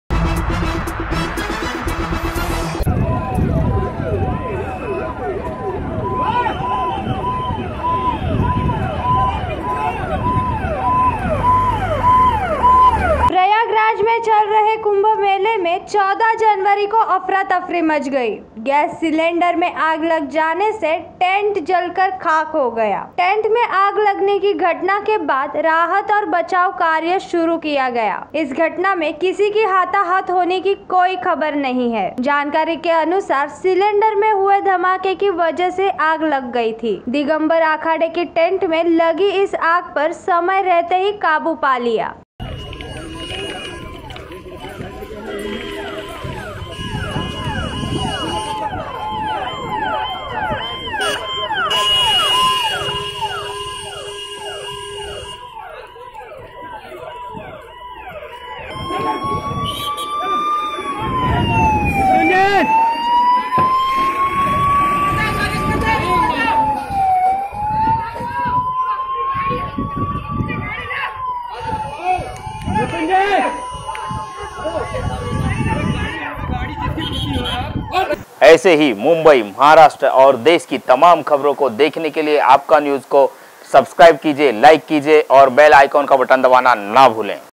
कुम्भ में १४ जनवरी को अफरातफरी मच गई || Aapka News
कुम्भ में १४ जनवरी को अफरातफरी मच गई
दिगम्बर अखाड़े के टेन्ट में लगी थी आग